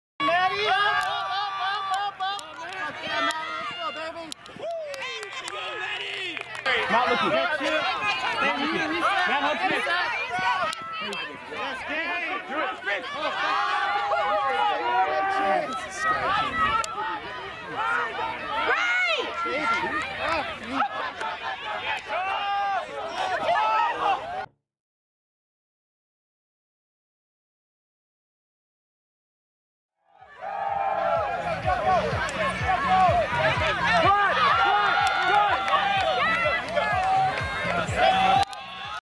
2018 PEC Love Tractor vs Public Enemy Highlights
Highlights from Denver Love Tractor versus Dallas Public Enemy in the first round of pool play at the 2018 USA Ultimate Pro Elite Challenge in Broomfield, CO on July 7.